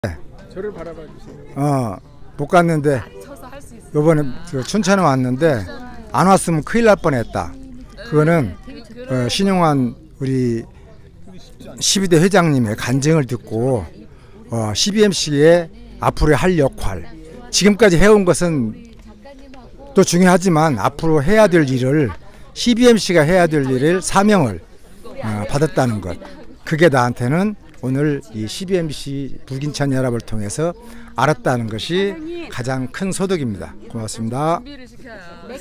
신용한회장님의 간증을 통해 앞으로 해야할 사명을 받았습니다. cbmc 부평지회 김백수대표.I received a mission to do in the future.
Through Chairman Shin Han's testimony, I received a mission to do in the future. Kim Baek-soo, CEO of cbmc Bupyeong branch.
신용한회장님의 간증을 통해 앞으로 해야할 사명을 받았습니다. cbmc 부평지회 김백수대표.